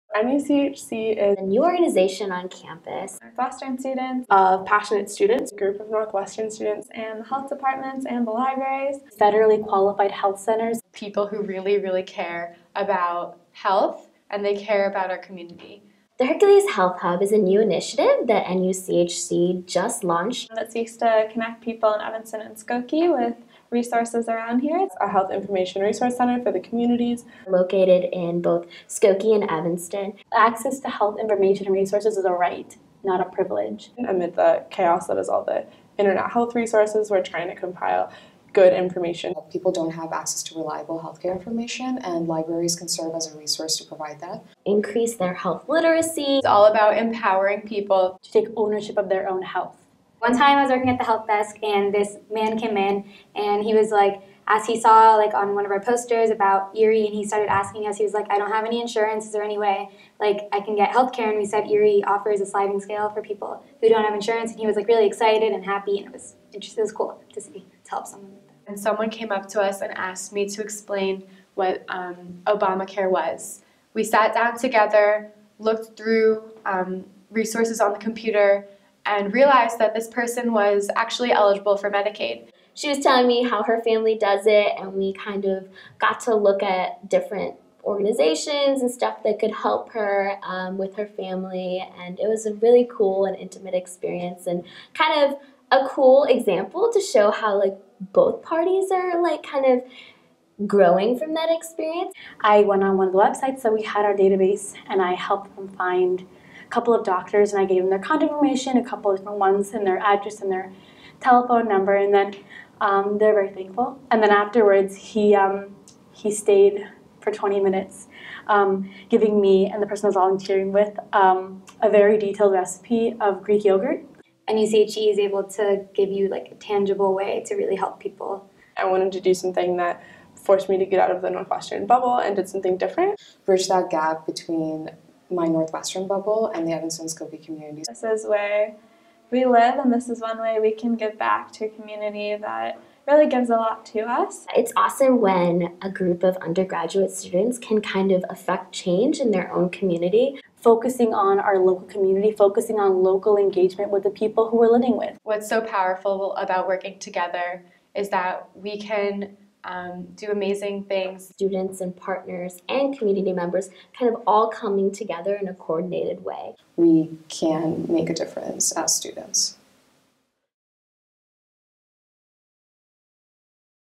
Northwestern Community Health Corps
The HIRCULES Health Desk is a health information desk in both Evanston and Skokie Public Libraries run by Northwestern Community Health Corps (NUCHC) volunteers.